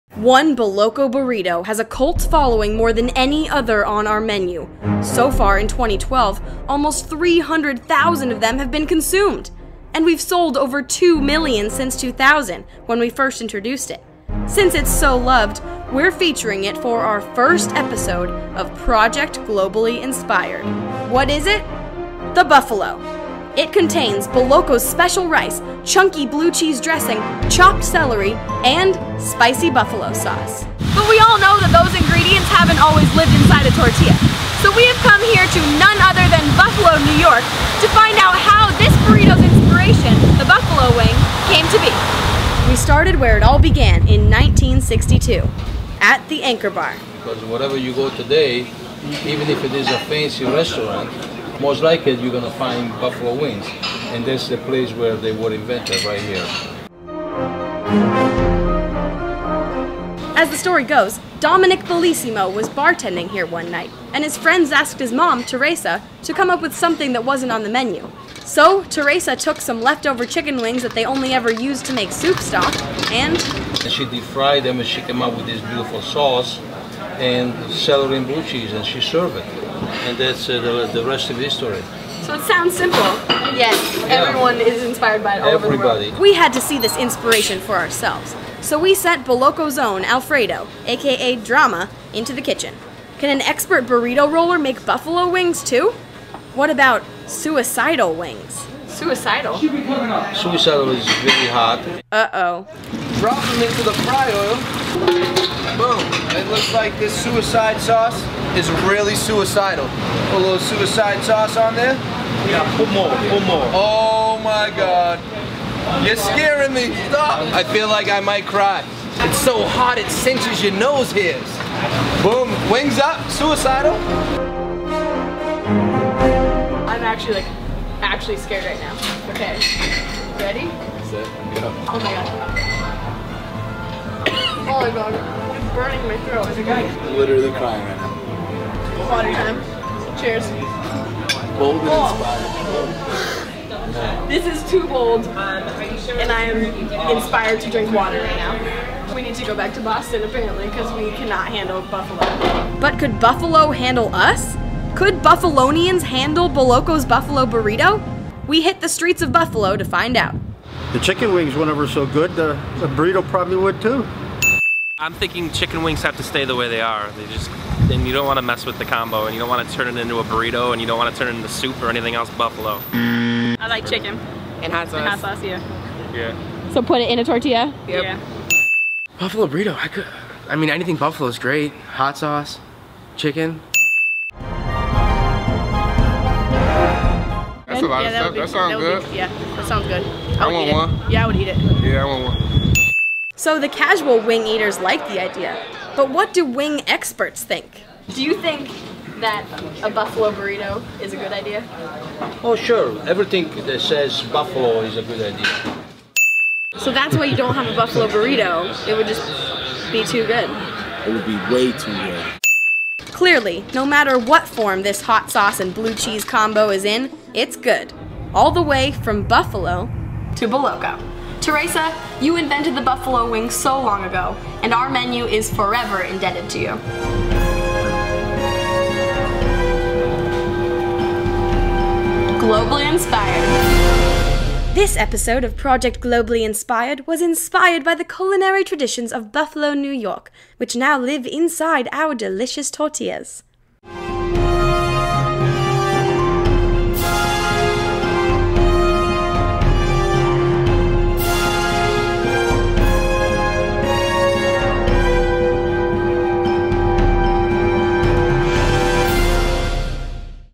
The Origin of the Buffalo Wing (and therefore, the Buffalo Burrito)
One Boloco burrito has a cult following more than any other on our menu... Watch this video to find out what it is... and where its original inspiration came from!

Then follow us to see where our next burrito adventures take us!